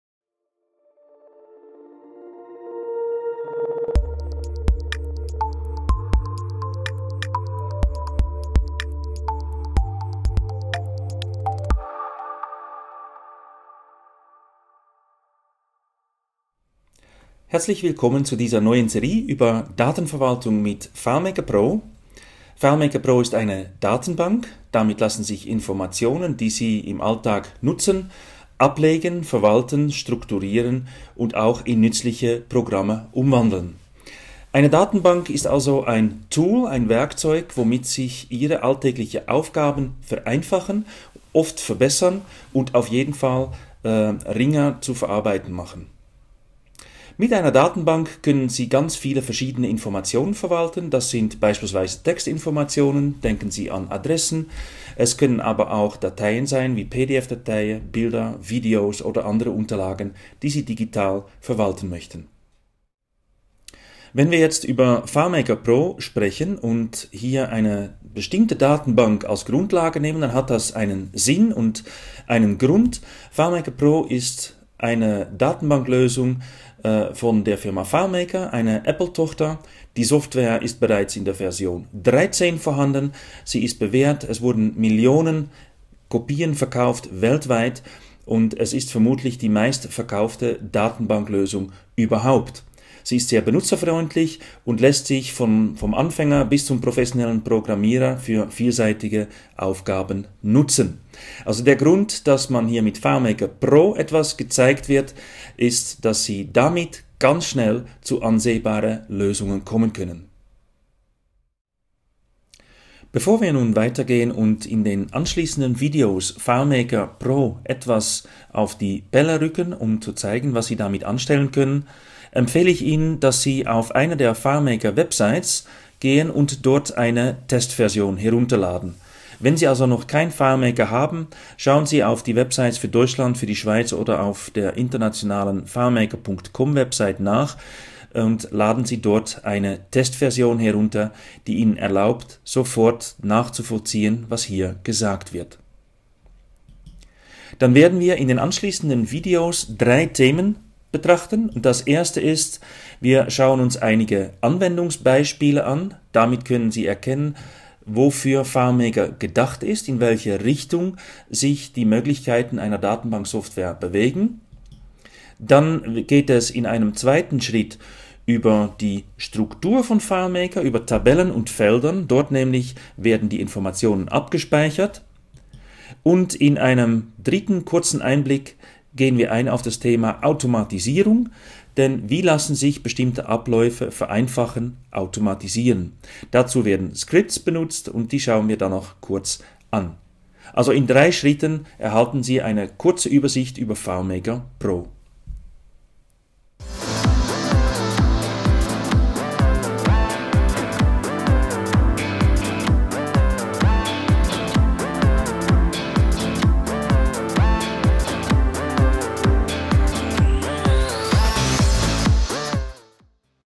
FileMaker Pro: Der generelle Überblick (c2p)
Dieses Video beschreibt, wozu eine Datenbank dient, und was Sie in den Videos der ersten Serie erwarten können.